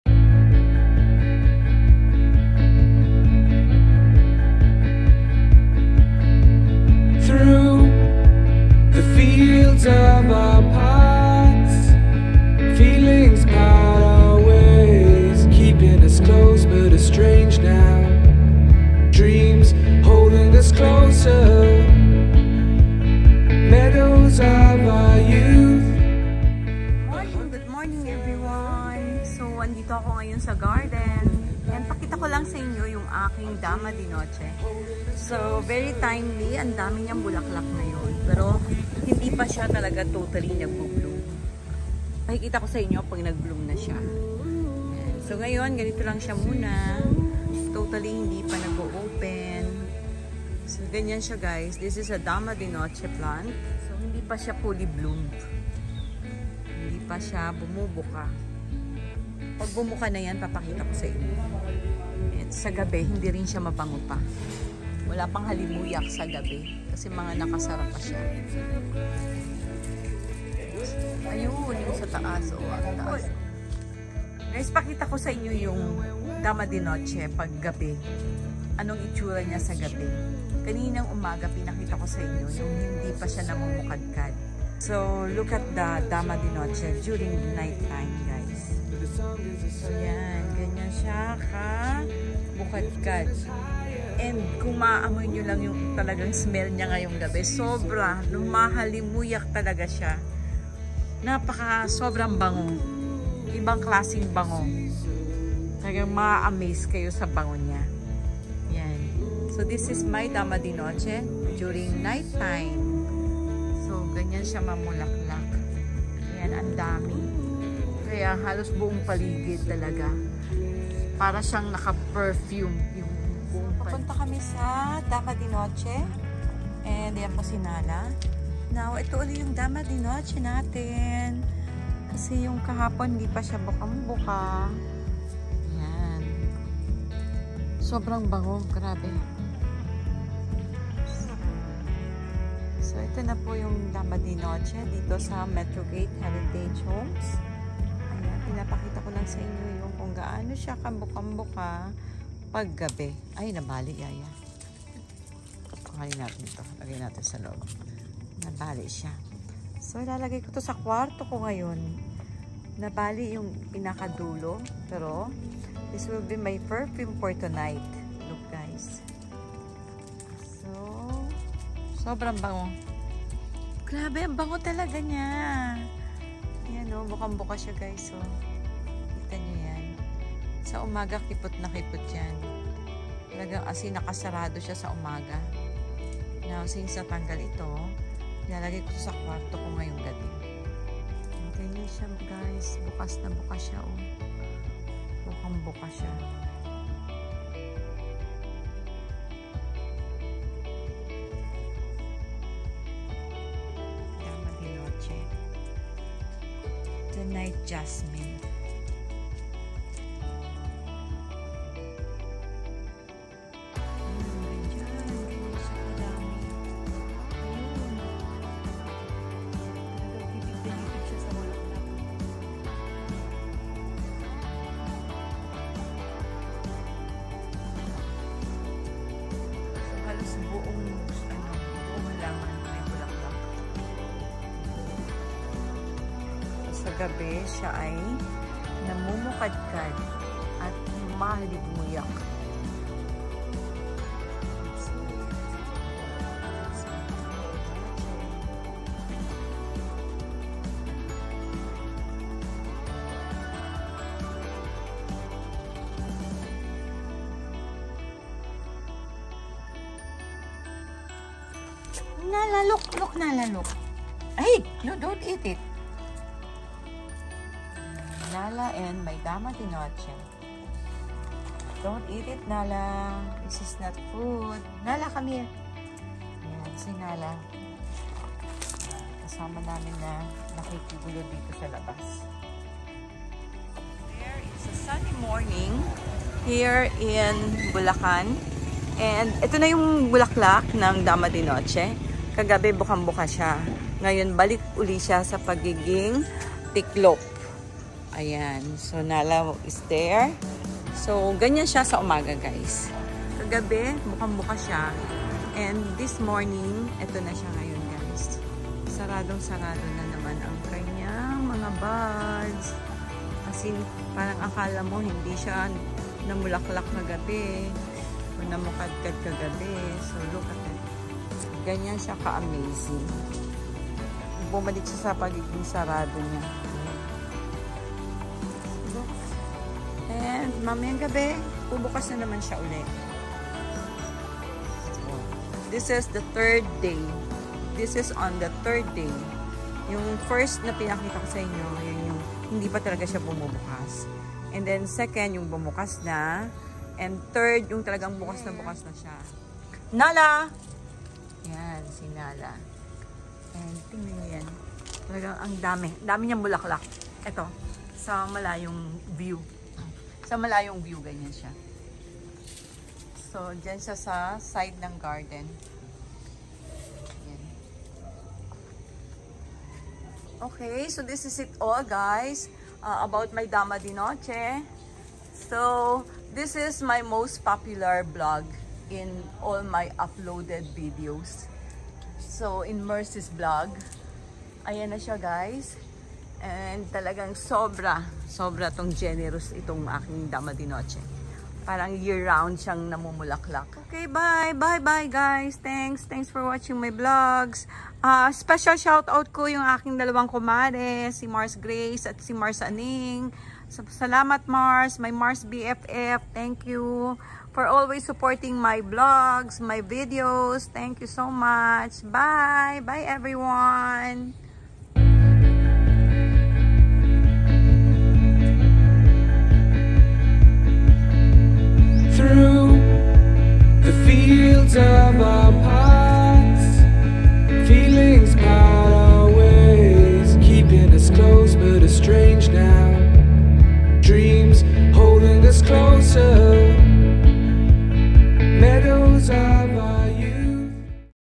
MERCY’S GARDEN: DAMA DE NOCHE ( THE NIGHT-BLOOMING JASMIN) TWO FACES...
I always love flowers and i grow them in my garden, one amazing bloom is my Dama de Noche, with its beautiful star-like appearance and the gracious fragrant scents.